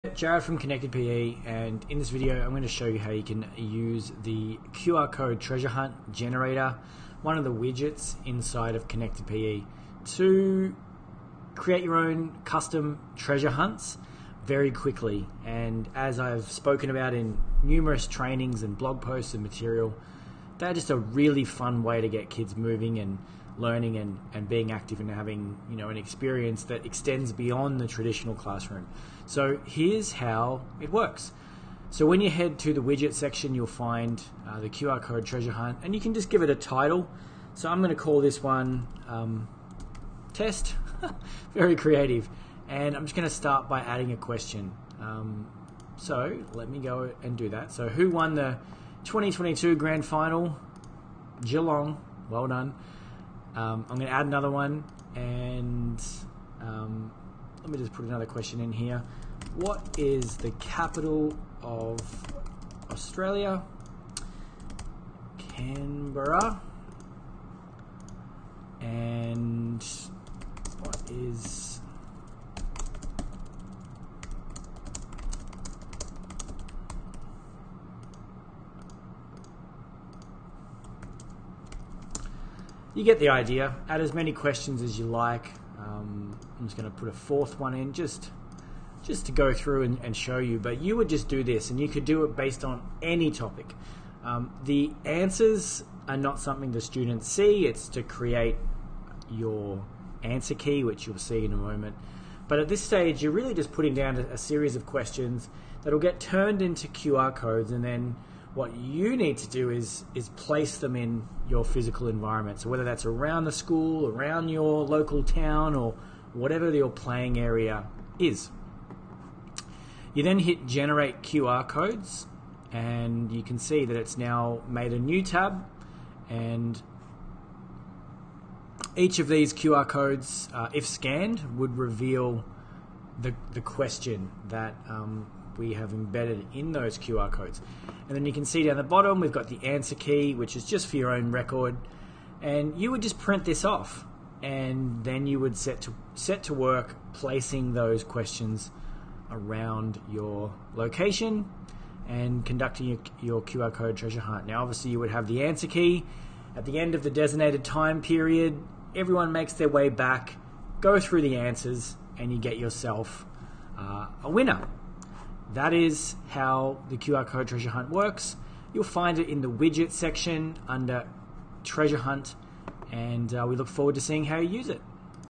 QR Code Treasure Hunt Generator
Welcome to our tutorial video on how to use the brand-new QR Code Treasure Hunt Generator feature on ConnectedPE! We are excited to show you how to turn your questions into an engaging treasure hunt, creating a fun and interactive learning experience for your students.

In this video, we'll take you step by step through the process of setting up your custom QR Code Treasure Hunt using our new widget. Whether you're aiming to create an orientation activity for new students, a fun revision task, or a brain-boosting activity, our QR Code Treasure Hunt Generator can help make it easier and more enjoyable.

Here's what you can expect to learn from this tutorial:

Navigating to the QR Code Treasure Hunt Generator on our platform.
Inputting your questions or tasks to create custom QR codes.
Printing and laminating your QR codes for durability.
Setting up your first QR Code Treasure Hunt!

We can't wait to hear about the creative ways you incorporate QR Code Treasure Hunts into your Physical Education classes.

Please leave any questions or feedback in the comment section below, and we'll be sure to respond.